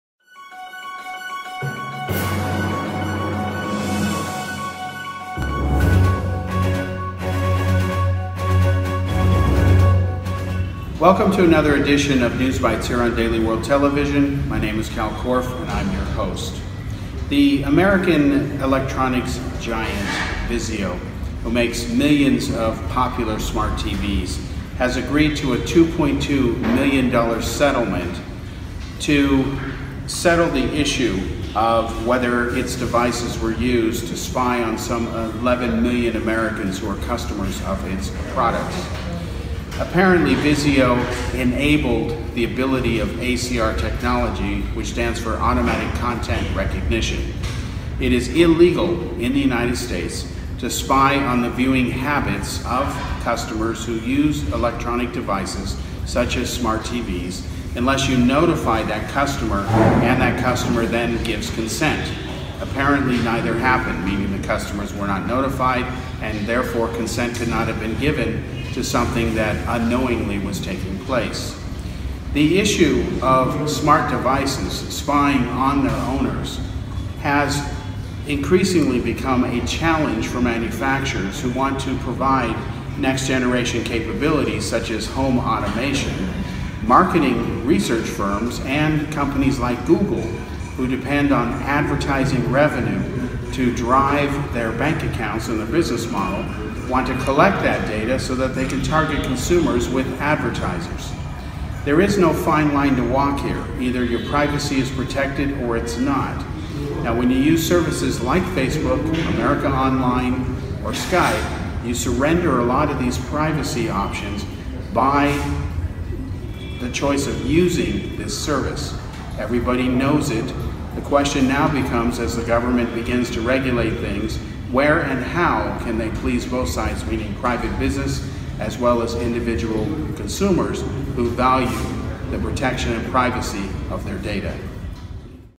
Vizio To Pay Millions for Spying on People
Vizio, the make of popular electronics such as "smart TVs,"has agreed to pay $2.2 million in damages for spying on some 11 million customers who bought its products by monitoring the content they watched, without either their knowledge or consent. Kal Korff comments.